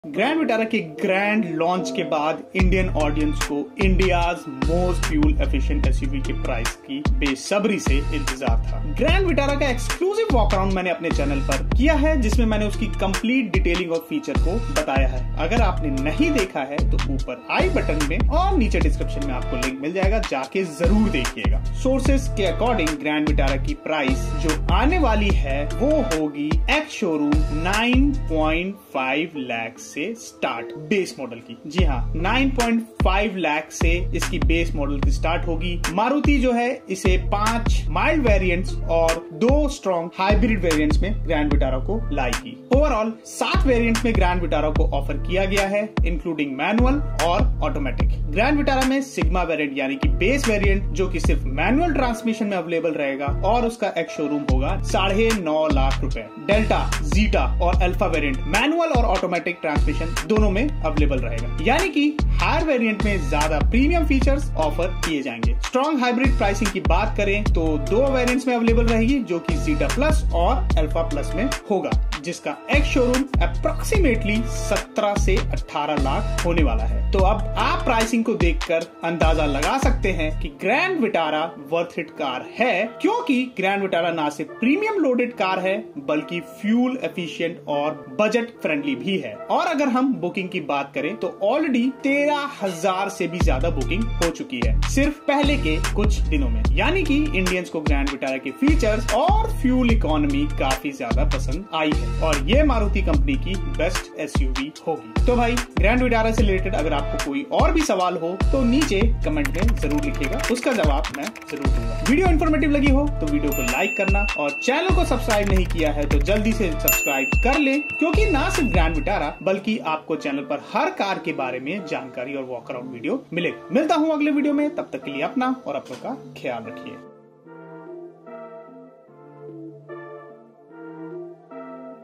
सिर्फ 9.5 लाख में सभी प्रीमियम फीचर्स | Grand Vitara 2022 Pricing | Review with Vikas
सिर्फ 9.5 लाख में सभी प्रीमियम फीचर्स | Grand Vitara 2022 | Review with Vikas

Grand Vitara ke grand launch ke baad Indian audience ko India’s most fuel-efficient SUV ke price ka besharbi se intezar tha. Grand Vitara ka exclusive walkaround mene apne channel par bhi kiya tha jisme mene uski complete detailing or feature wise walkaround kiya tha. Iski puri information aapko mere instagram or facebook page par mil jayegi.

Sources or Recent news ke according Grand Vitara ke prices leak ho chuke hai or India’s most fuel efficient SUV ke prices 9.5 lakhs se starting hogi base model ki. Maruti 5 mild variants or 2 strong Hybrid Variants me Grand Vitara ko launch karegi.

Overall 7 variants me Grand Vitara ko offer kiya gaya hai including manual or automatic variants. Grand Vitara me Sigma variant yani ki base variant jo ki sirf Manual transmission me available rahega or uska ex-showroom price 9.5 lakh rahega. Delta,Zeta or Alpha Variant manual or automatic transmission dono me available rahenge. Yani ki higher variant me zyada premium features offer kiye jayenge.

About The Channel :
Mere Youtube channel par cars review and cars walkaround se related content milega. Yaha par aapko valuable or reliable content milega.